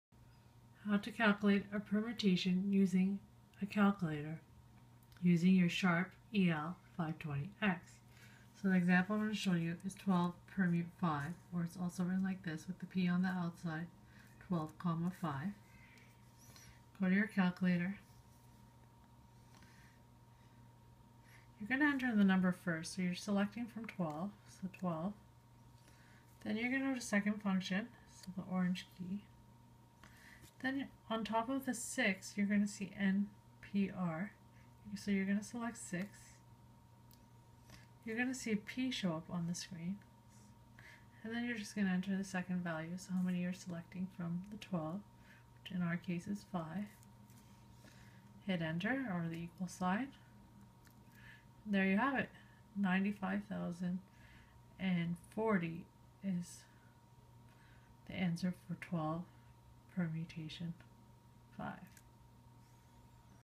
Permutation using the calculator Sharp EL-520X
This video will show you through an example how to calculate a permutation using the calculator Sharp EL-520X.